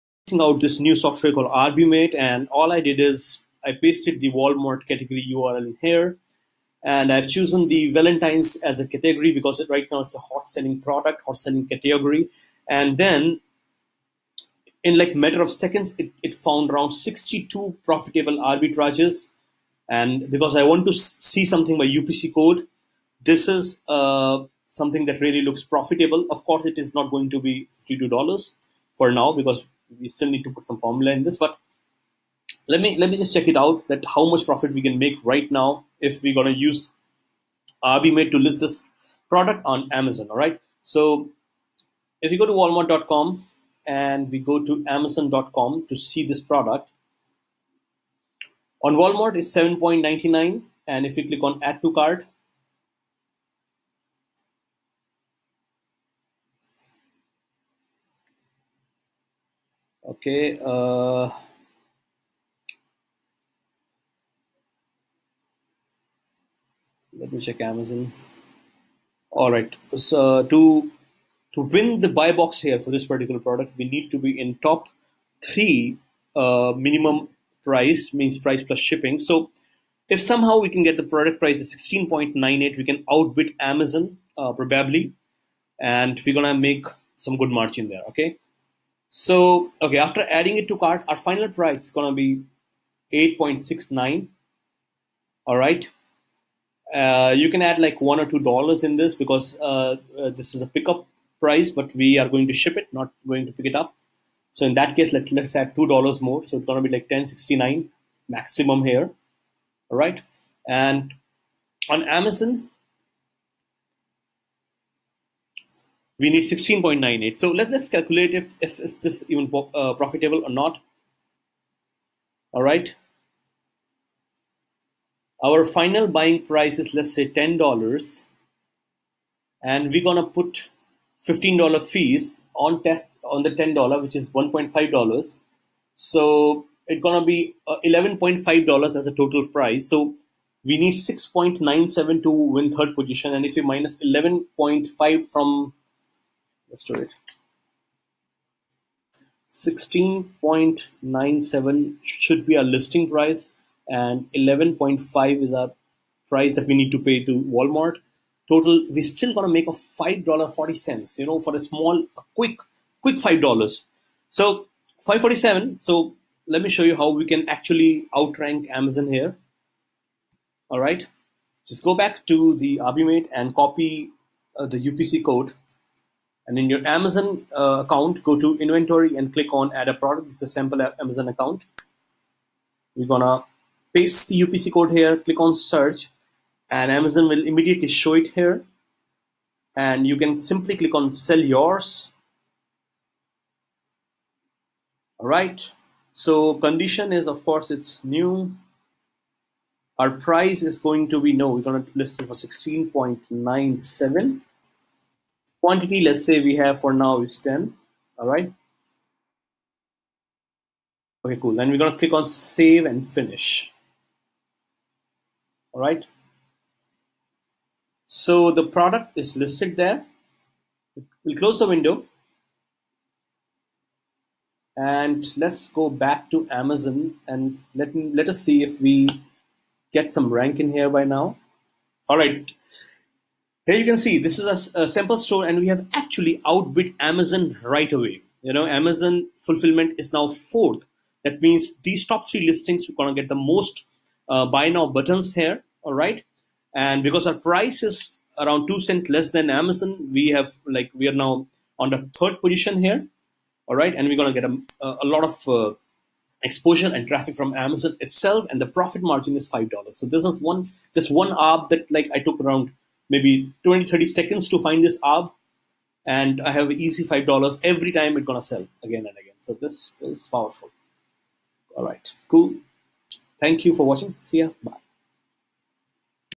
arbiMate Case Study 01 - Finding and listing products
arbiMate is a unique product that simplifies WalmarttoAmazonarbitrages. Let's take a quick look at this tool and understand how you can profit from it

Arbimate is one of the best automated arbitragefinder. Here is walkthrough of this tool.

To start with paste the walmart category url as shown. You will find profitable arbitrages in a matter of seconds. Take a quick look at the listings, pick the one where there is a good margin.

Fix a price that will outbid Amazon and start selling right away. Arbs are a surefire way to make money and Arbimate will make it even more easier for you.

to know more.